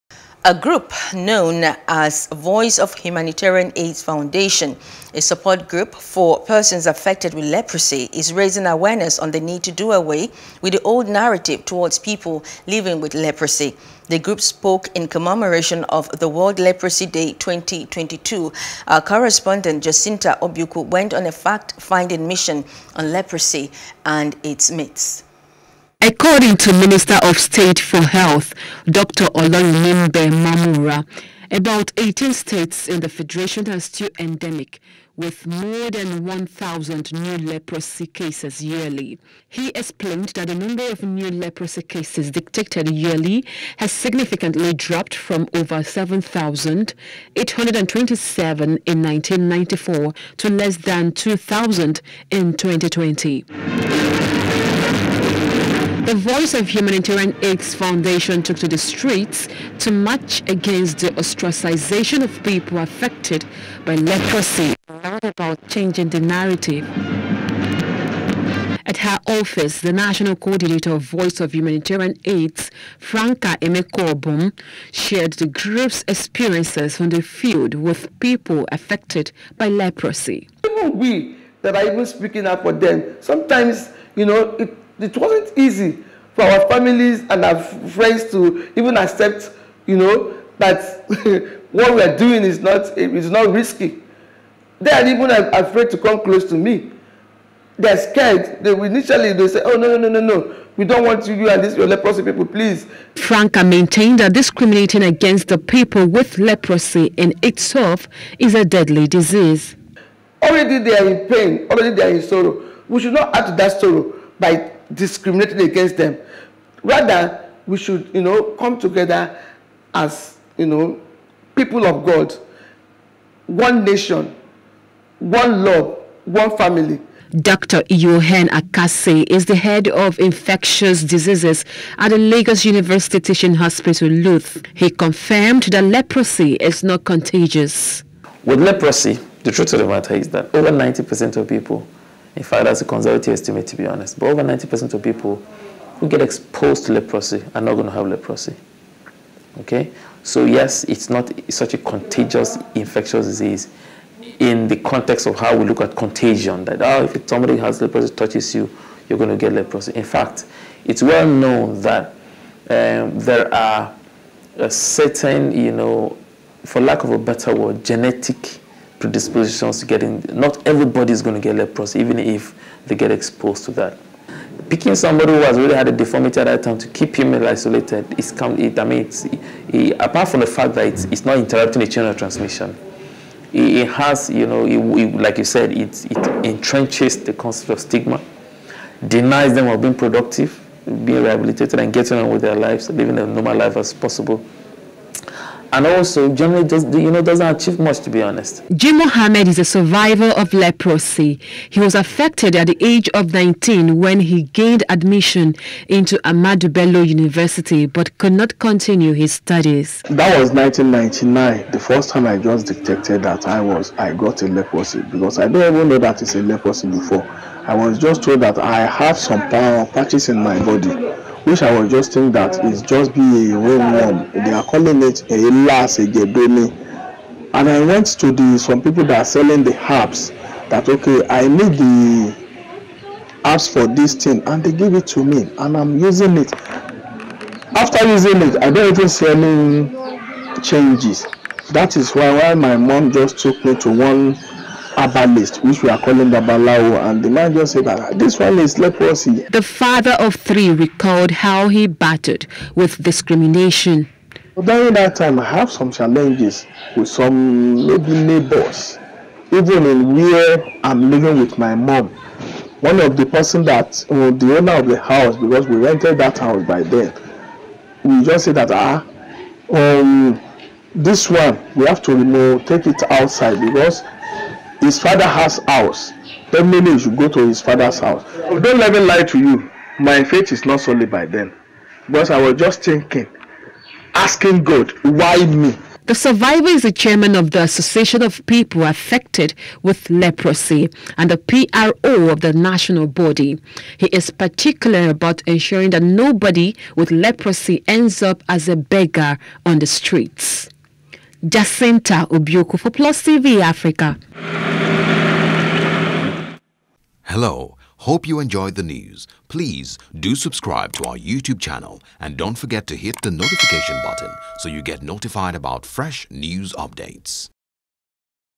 Health: Leprosy Is Curable, People Affected Are Not To Be Isolated Anymore -Expert | NEWS
A group known as voice of humanitarian aids foundation, a support group for the persons affected with Leprosy is raising awareness on the need to do away with the old narrative towards people with Leprosy.